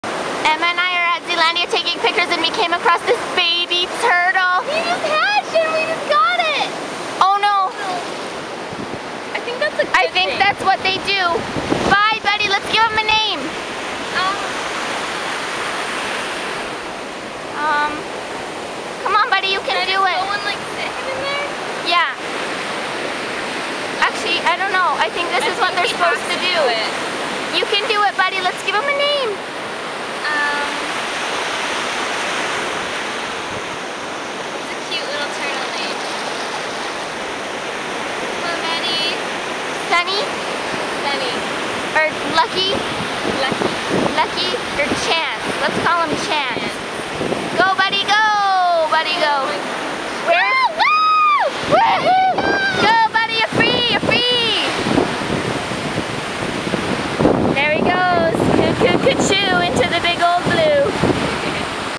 Koo Koo Ka Choo
Emma and Hannah, by chance, got to watch a baby sea turtle make its way into the 'Big Ole Blue.'